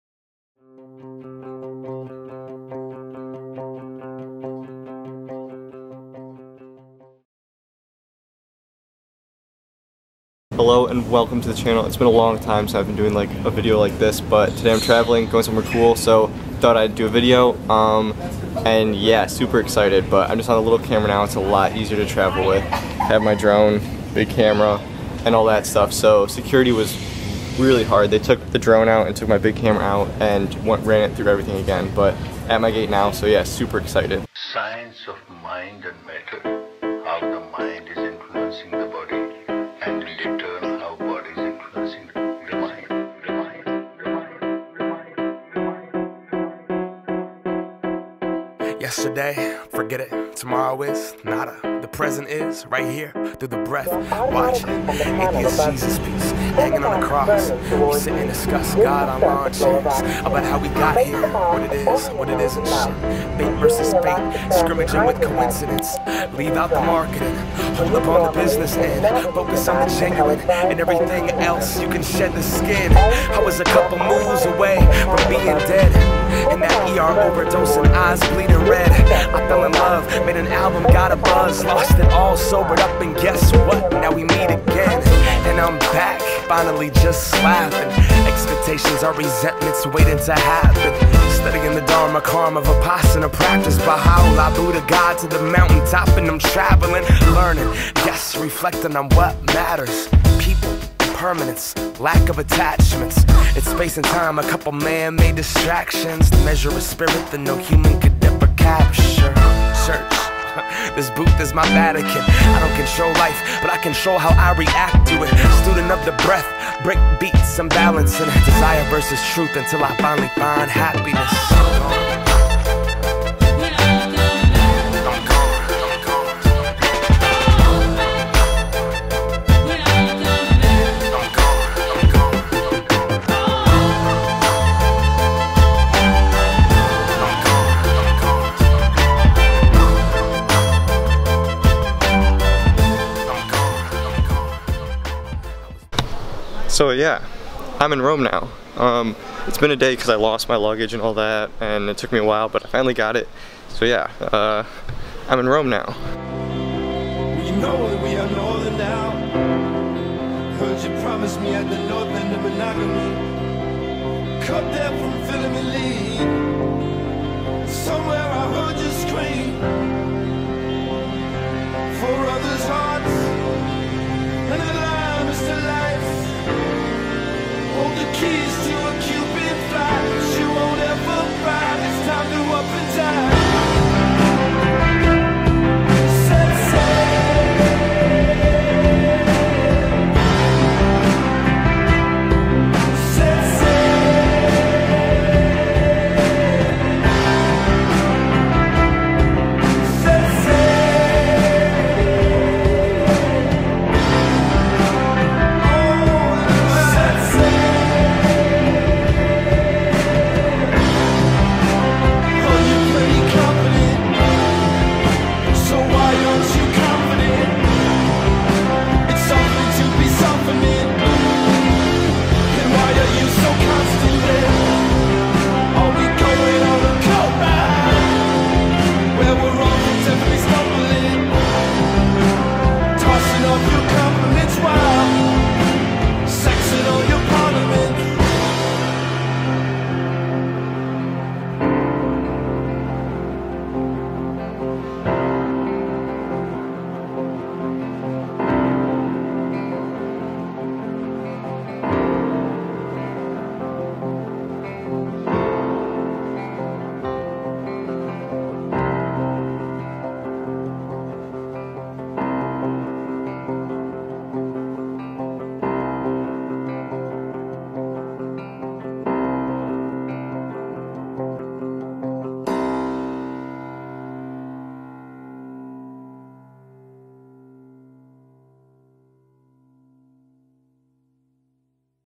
I left the USA
In this video... I share the story of my trip to Europe. It took me a while to upload this because I haven't had a way to charge my Mac for a while, but now I'm back.